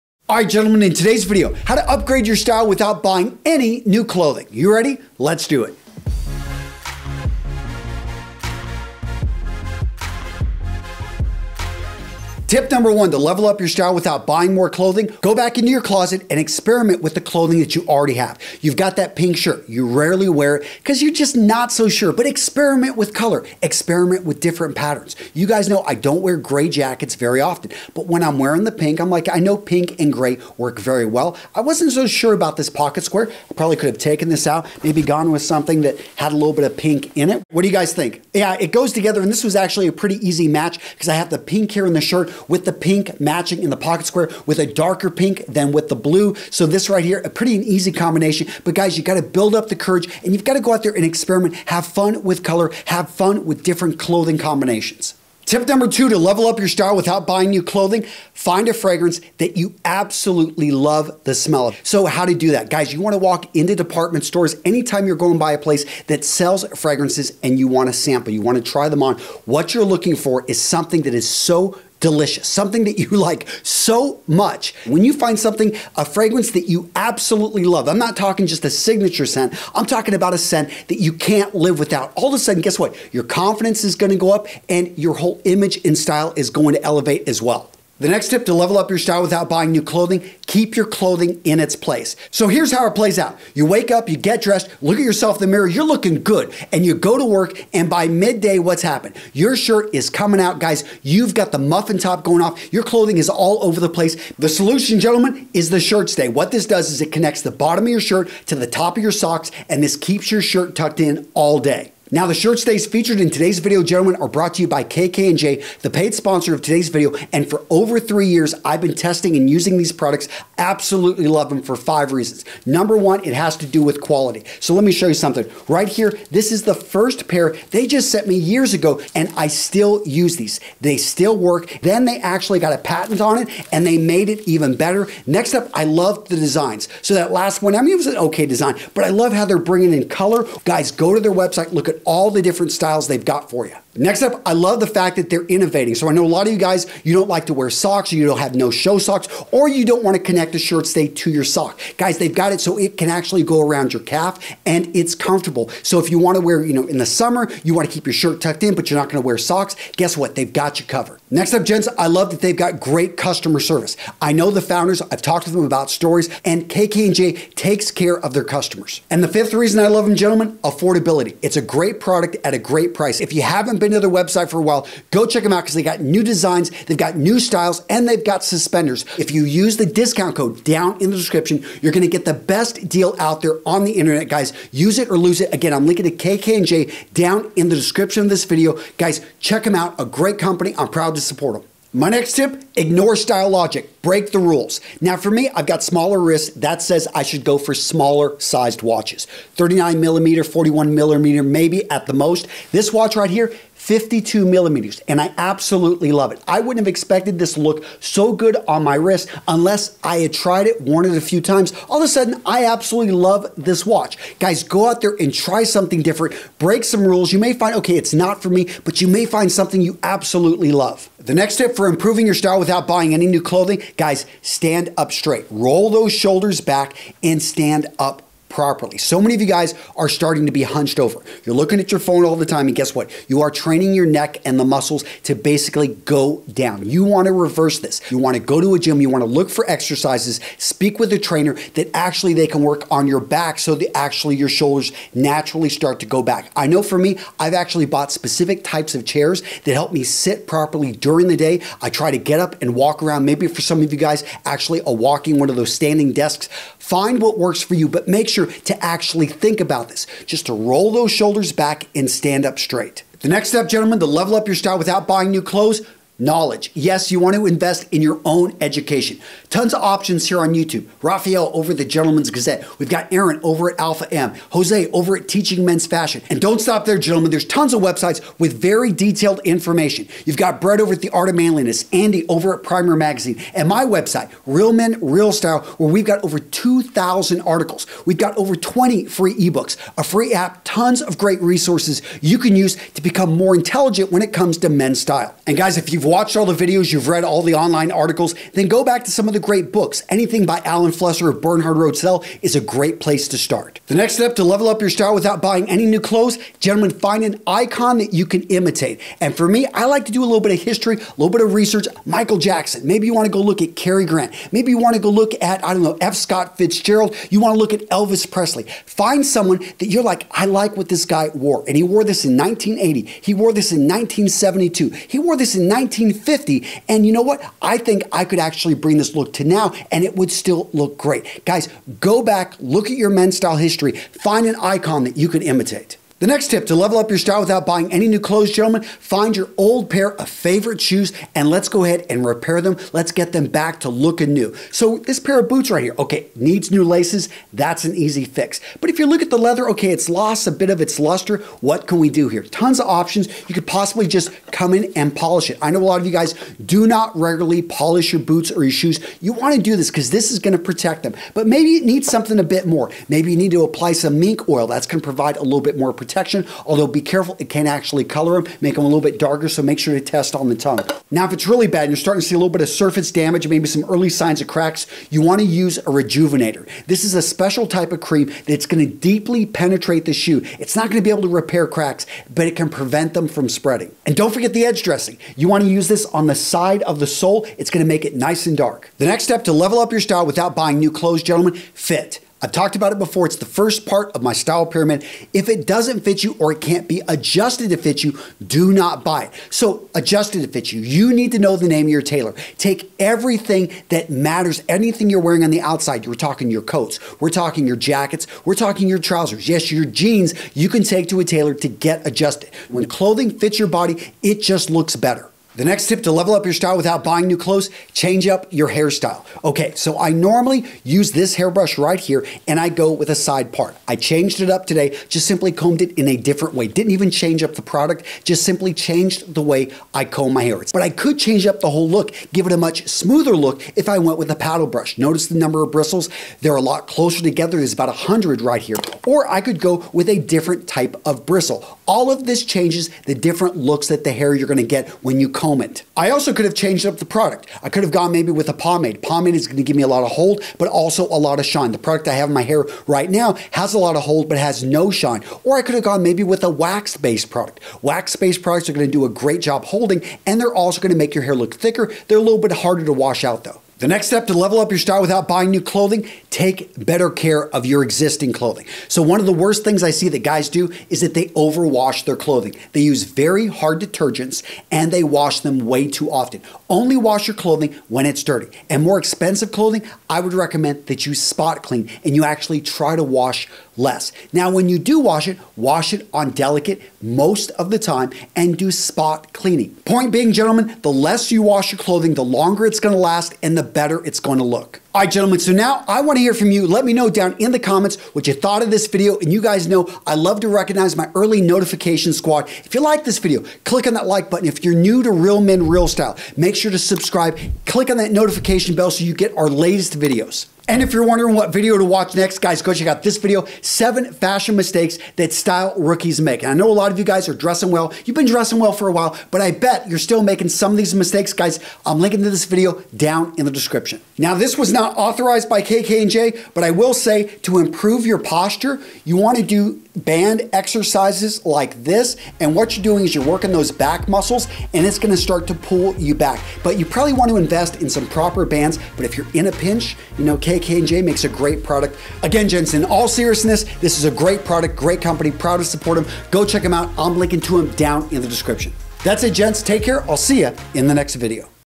How To Dress Sharp WITHOUT Buying NEW Clothes (Inexpensive Style Improvement Tips) RMRS Style Videos
Video Summary:

0:15 - Experiment with items you already own
0:57 - Find a fragrance you absolutely love
1:31 - Keep your clothing in place
3:31 - Defy logic and break some style rules
3:46 - Improve your posture
4:26 - Increase your style knowledge
5:05 - Find an icon to imitate
5:37 - Restore an old pair of shoes or boots
6:32 - Get your clothing tailored
7:00 - Change up your hairstyle
7:47 - Take better care of your clothing

📫 Shipping Address

Antonio Centeno
USA

PRODUCTS

RECOMMENDED PRODUCTS